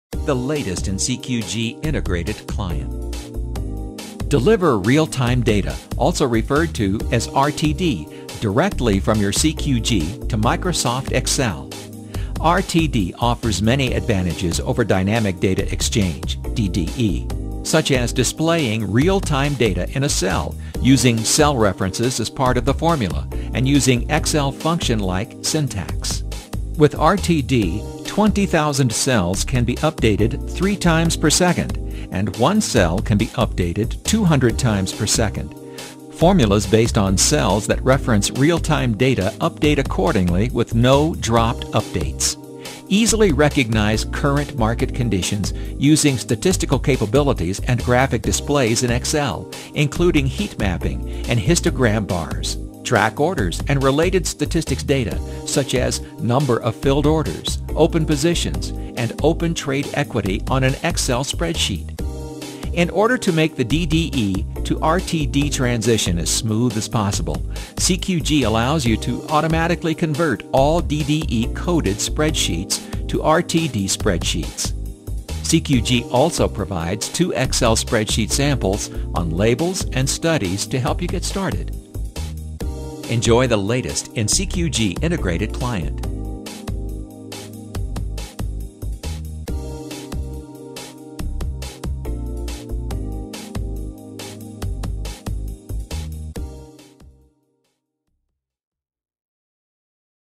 Real-Time Data (RTD)
Deliver RealTimeData (RTD) directly from your CQG to Microsoft Excel®. RTD offers many advantages over dynamic data exchange (DDE), such as displaying real-time data in a cell, using cell references as part of the formula, and using Excel function-like syntax. With RTD, 20,000 cells can be updated 3 times per second, and 1 cell can be updated 200 times per second. Formulas based on cells that reference real-time data update accordingly with no dropped updates. Let RTD efficiently track the state and location of real-time data formulas.